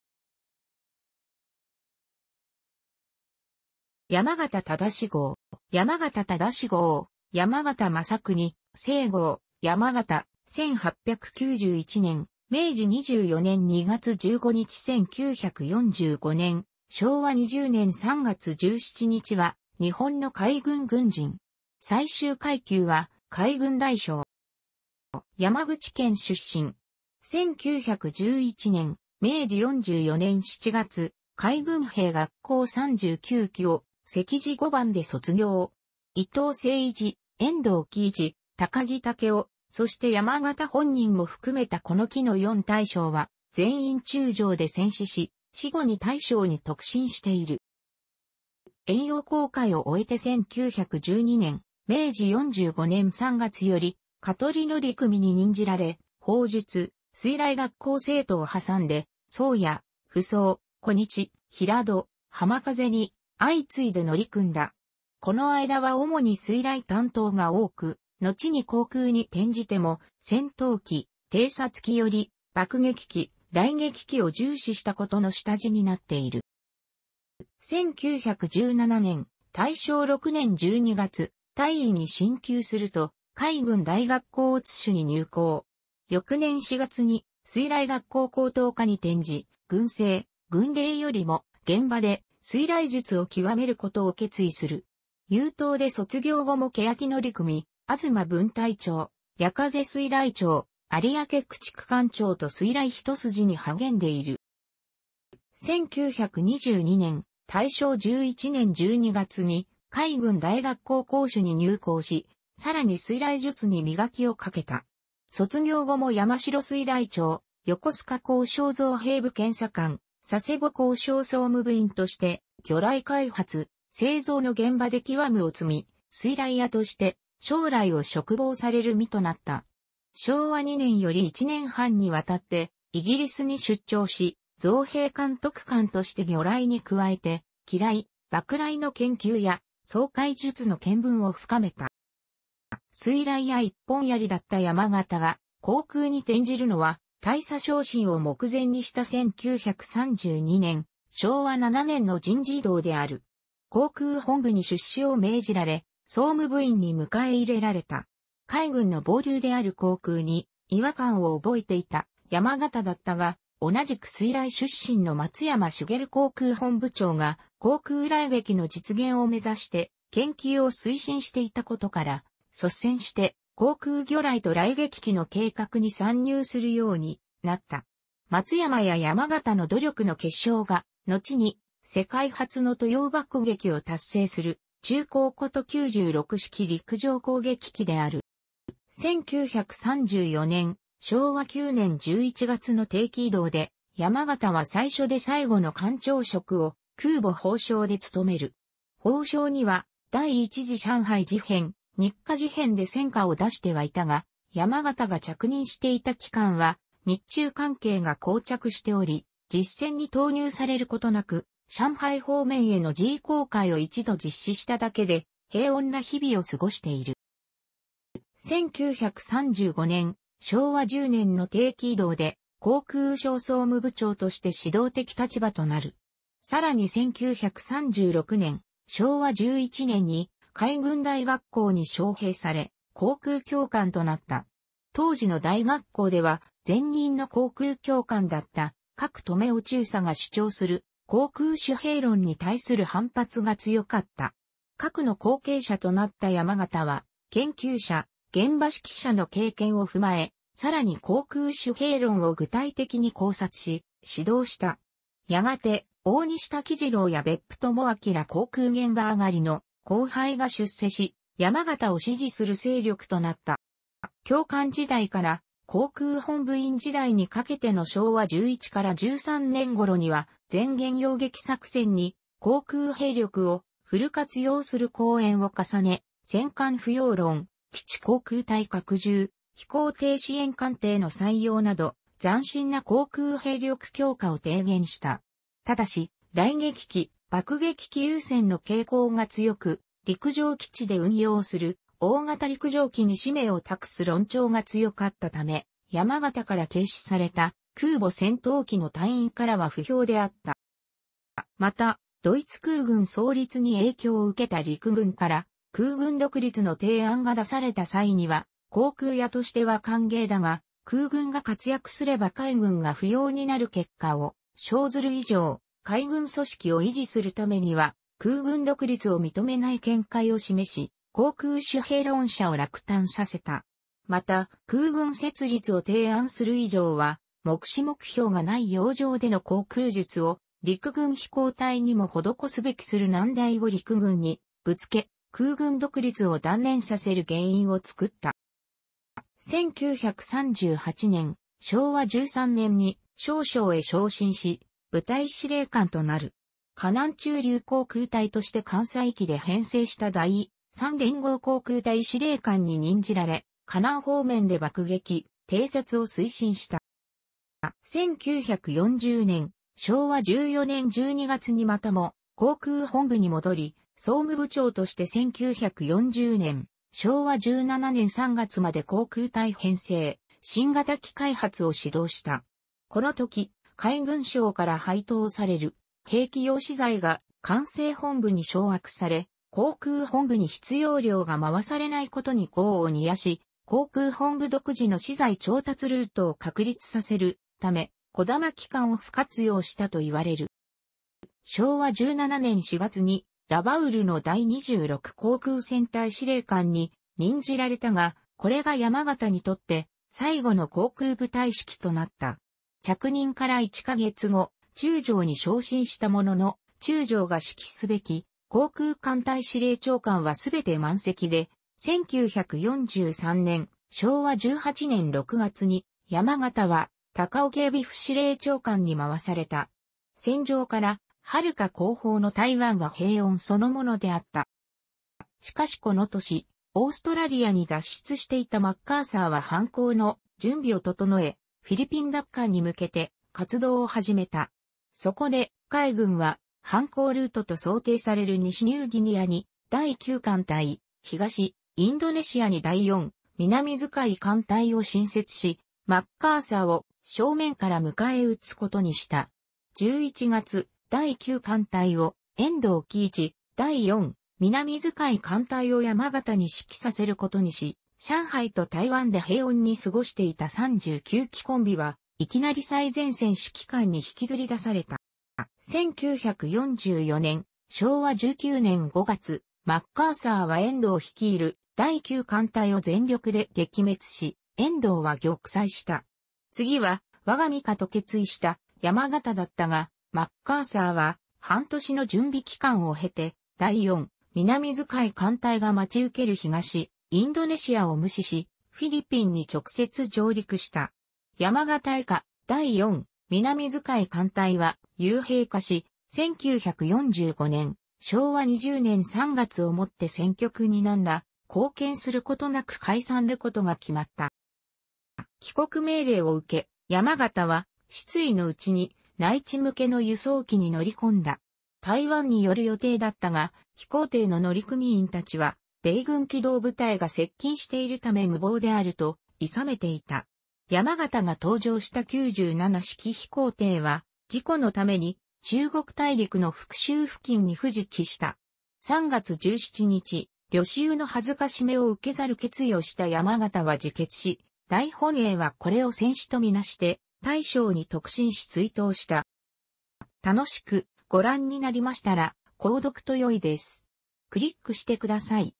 山縣正郷
山縣 正郷（やまがた まさくに、せいごう、山県、1891年（明治24年）2月15日 ‐ 1945年（昭和20年）3月17日）は、日本の海軍軍人。最終階級は海軍大将。

山口県出身。1911年（明治44年）7月、海軍兵学校39期を席次5番で卒業。伊藤整一、遠藤喜一、高木武雄、そして山縣本人も含めたこの期の4大将は全員中将で戦死し、死後に大将に特進している。

遠洋航海を終えて1912年（明治45年）3月より香取乗組に任じられ、砲術・水雷学校生徒を挟んで宗谷・扶桑・子日・平戸・浜風に相次いで乗組んだ。この間は主に水雷担当が多く、のちに航空に転じても戦闘機・偵察機より爆撃機・雷撃機を重視したことの下地になっている。

1917年（大正6年）12月、大尉に進級すると海軍大学校乙種に入校。翌年4月に水雷学校高等科に転じ、軍政・軍令よりも現場で水雷術を極めることを決意する。優等で卒業後も欅乗組・吾妻分隊長・矢風水雷長・有明駆逐艦長と水雷一筋に励んでいる。

1922年（大正11年）12月に海軍大学校甲種に入校し、さらに水雷術に磨きをかけた。卒業後も山城水雷長・横須賀工廠造兵部検査官・佐世保工廠総務部員として魚雷開発・製造の現場で研究を積み、水雷屋として将来を嘱望される身となった。昭和2年より1年半にわたってイギリスに出張し、造兵監督官として魚雷に加えて機雷・爆雷の研究や掃海術の見聞を深めた。

水雷屋一本槍だった山縣が航空に転じるのは、大佐昇進を目前にした1932年（昭和7年）の人事異動である。航空本部に出仕を命じられ、総務部員に迎え入れられた。海軍の傍流である航空に違和感を覚えていた山縣だったが、同じく水雷出身の松山茂航空本部長が航空雷撃の実現を目指して研究を推進していたことから、率先して航空魚雷と雷撃機の計画に参入するようになった。松山や山縣の努力の結晶が、のちに世界初の渡洋爆撃を達成する「中攻」こと96式陸上攻撃機である。

1934年（昭和9年）11月の定期異動で、山縣は最初で最後の艦長職を空母鳳翔で勤める。鳳翔には第一次上海事変・日華事変で戦果を出してはいたが、山縣が着任していた期間は日中関係が膠着しており、実戦に投入されることなく、上海方面への示威航海を一度実施しただけで平穏な日々を過ごしている。

1935年（昭和10年）の定期異動で航空廠総務部長として指導的立場となる。さらに1936年（昭和11年）に海軍大学校に招聘され、航空教官となった。当時の大学校では、前任の航空教官だった加来止男中佐が主張する航空主兵論に対する反発が強かった。加来の後継者となった山縣は、研究者・現場指揮者の経験を踏まえ、さらに航空主兵論を具体的に考察し、指導した。やがて大西瀧治郎や別府朋明ら航空現場上がりの後輩が出世し、山縣を支持する勢力となった。

教官時代から航空本部員時代にかけての昭和11-13年頃には、漸減邀撃作戦に航空兵力をフル活用する講演を重ね、戦艦不要論・基地航空隊拡充・飛行艇支援艦艇の採用など、斬新な航空兵力強化を提言した。ただし、雷撃機・爆撃機優先の傾向が強く、陸上基地で運用する大型陸上機に使命を託す論調が強かったため、山縣から軽視された空母戦闘機の隊員からは不評であった。

また、ドイツ空軍創立に影響を受けた陸軍から空軍独立の提案が出された際には、航空屋としては歓迎だが、空軍が活躍すれば海軍が不要になる結果を生ずる以上、海軍組織を維持するためには空軍独立を認めない見解を示し、航空主兵論者を落胆させた。また、空軍設立を提案する以上は、目視目標がない洋上での航空術を陸軍飛行隊にも施すべきとする難題を陸軍にぶつけ、空軍独立を断念させる原因を作った。

1938年（昭和13年）に少将へ昇進し、部隊司令官となる。華南駐留航空隊として艦載機で編制した第3連合航空隊司令官に任じられ、華南方面で爆撃・偵察を推進した。

1940年（昭和14年）12月にまたも航空本部に戻り、総務部長として1940年（昭和17年）3月まで航空隊編制・新型機開発を指導した。この時、海軍省から配当される兵器用資材が艦政本部に掌握され、航空本部に必要量が回されないことに業を煮やし、航空本部独自の資材調達ルートを確立させるため「児玉機関」をフル活用したといわれる。

昭和17年4月にラバウルの第26航空戦隊司令官に任じられたが、これが山縣にとって最後の航空部隊指揮となった。着任から1ヵ月後、中将に昇進したものの、中将が指揮すべき航空艦隊司令長官はすべて満席で、1943年（昭和18年）6月に山縣は高雄警備府司令...